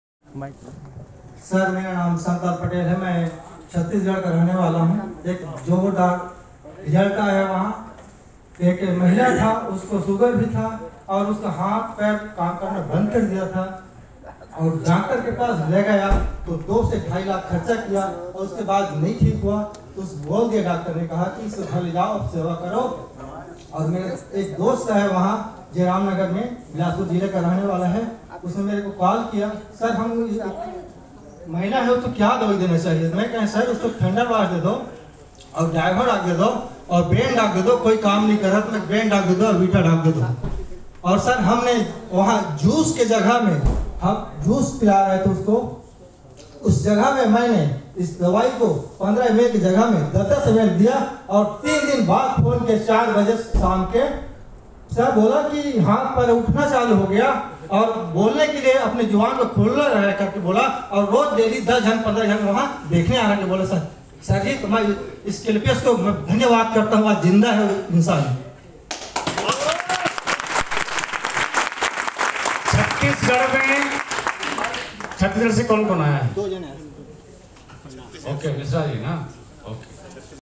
शुगर जैसे बीमारी में|2 से 4 लाख खर्च होने के बाद भी ठीक नही हुआ|तब Awpl ने ठीक किया कम
Blood_Pressure और
शुगर जैसे बीमारी में
Awpl ने अपना झंडा लहराया |
shugar ka result